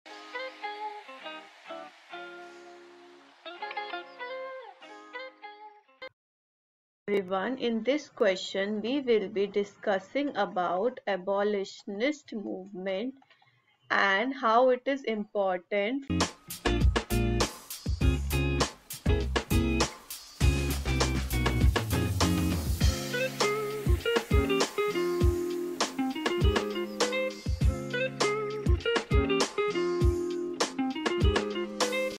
1. Why does Angelina GrimkÃ© call the abolitionist movement the nation's foremost "school of human …
1. Why does Angelina GrimkÃ© call the abolitionist movement the nation x27;s foremost quot;school of human rights quot;? 2. What role does she think the difference between the sexes should play in determining a person x27;s rights and obligations?